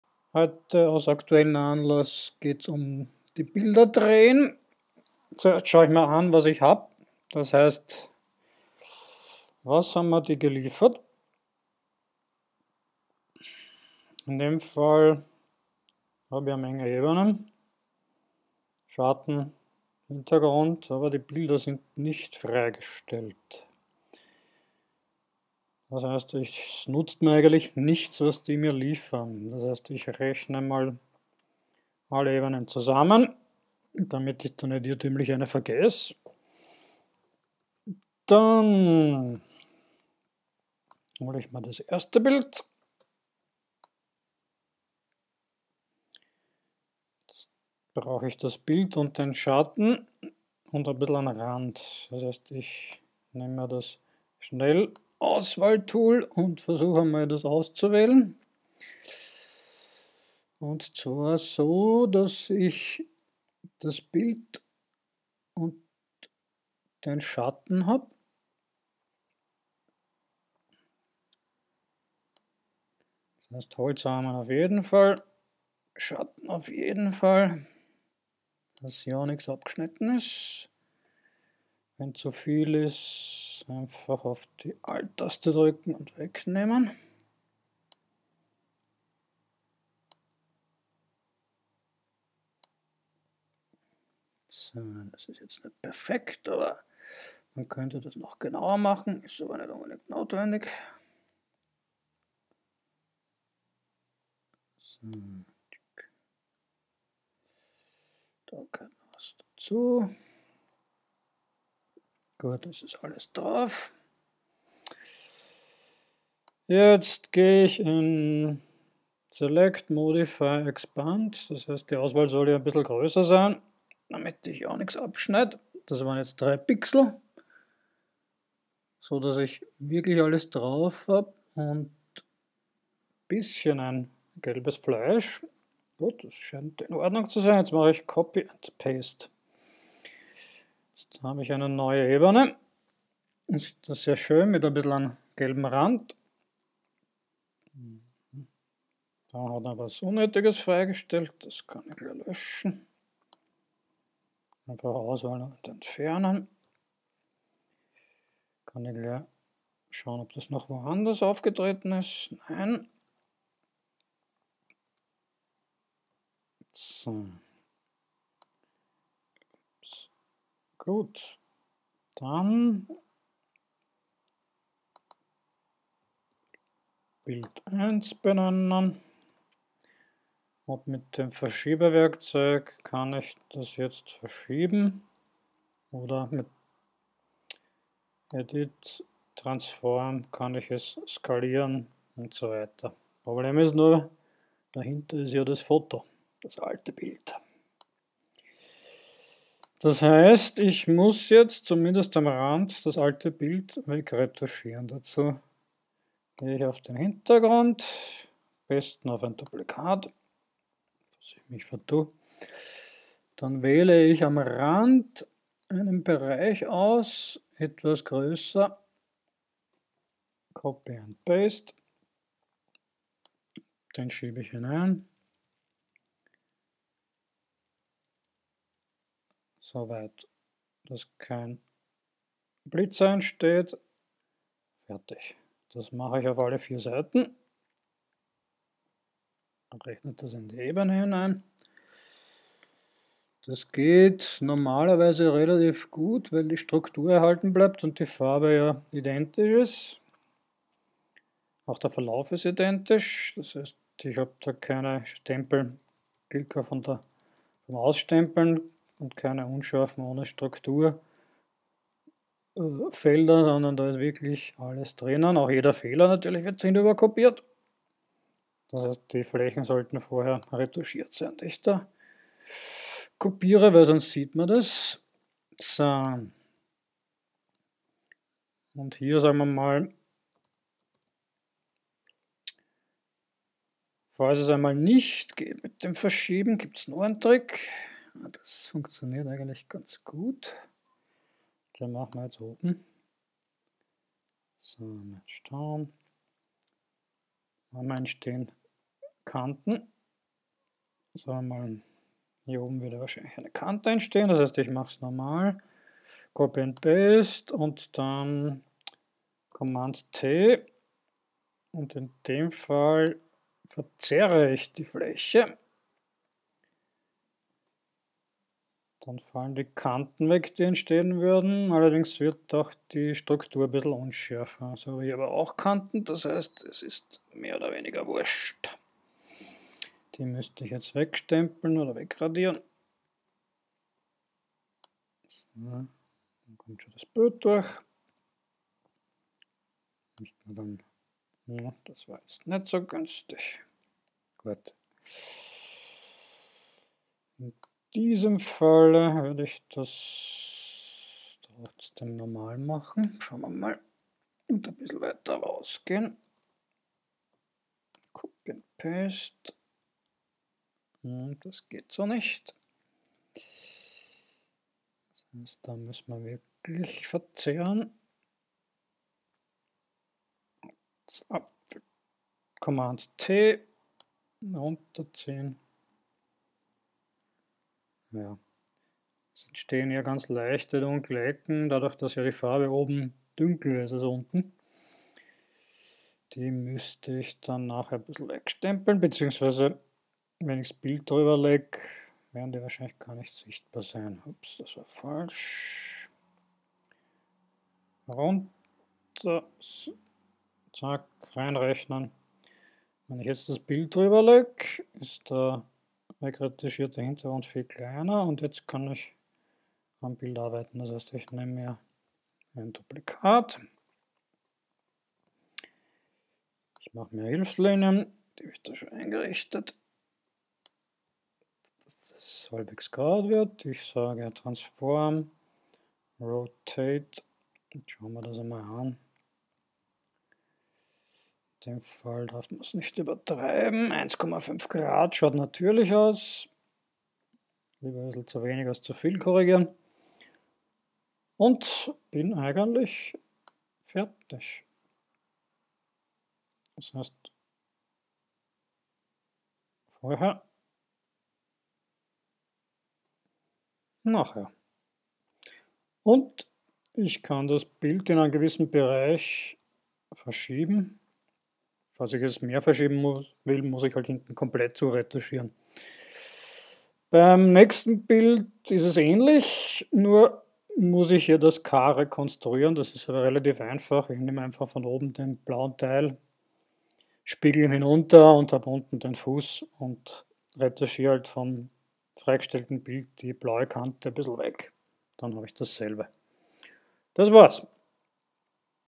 photoshop lektion 5
bilder aus dem hintergrund lösen und neu ausrichten